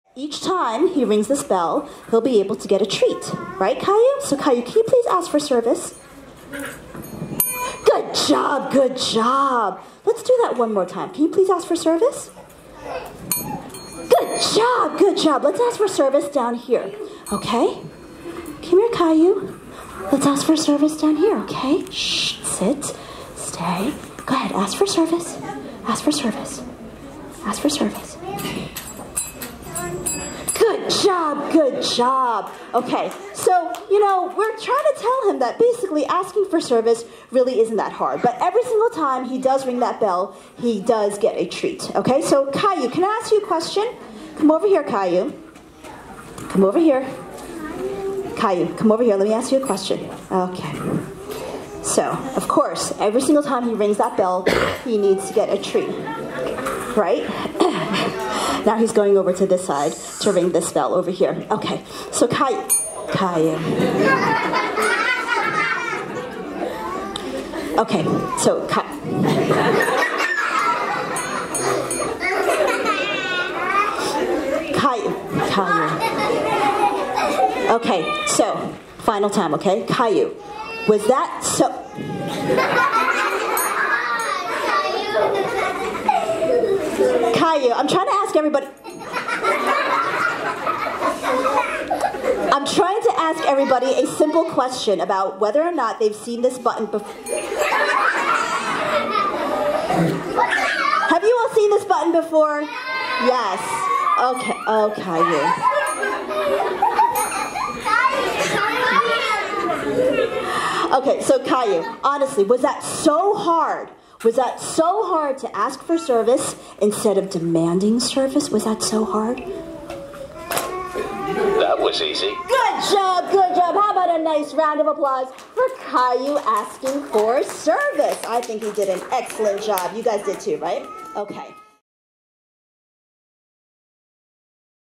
American Eskimo Dog's comedy routine
Kayu's comic relief during our dog tricks show at the Wall Township Library in NJ - November 2017